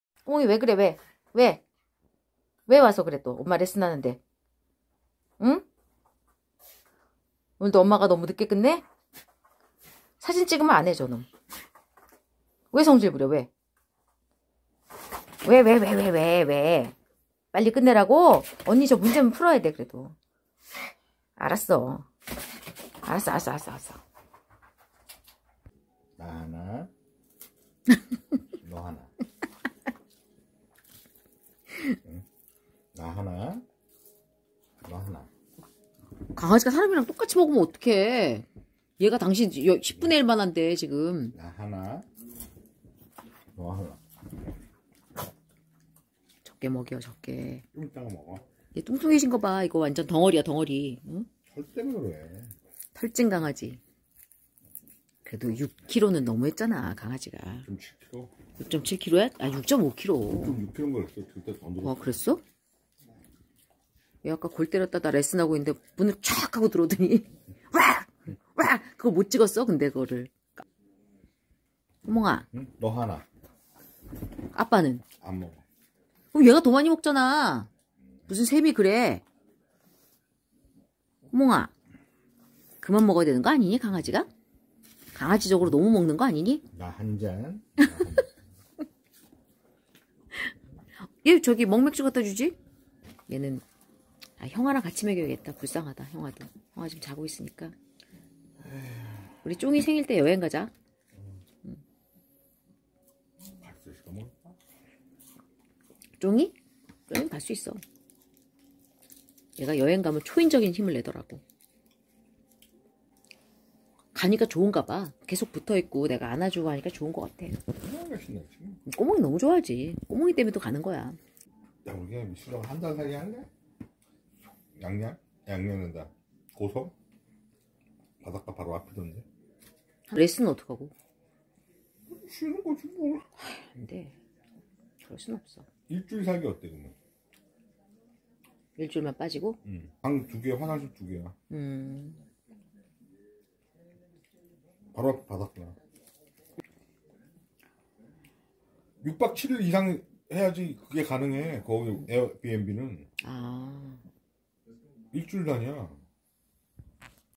꼬몽이의행패 시간지났잖아 행패부리는페키니즈 페키니즈성깔 시간이정확한페키니즈 레슨왜안끝내 먹태에진심인강아지 바닷가한달살기할까
오늘도 꼬몽이는 레슨이 늦어진다고 성질을 부리면서 방에 들어와 짖습니다. 레슨이 시간보다
길어지면 가끔 들어와서 성질을 부려요ㅋㅋ
시간이 어찌나 정확하신지 ㅋㅋ

저녁을 먹고 맥주 한 잔 하는 돌프님을 졸졸 쫒아 다니던 꼬몽이는 먹태도 나눠 먹고 있네요..  철없는 돌프님은 바닷가 한 달 살기를 하자는데ㅋㅋㅋㅋ결국 쫑이 생일 때 2박3일로 끝내기로 했네요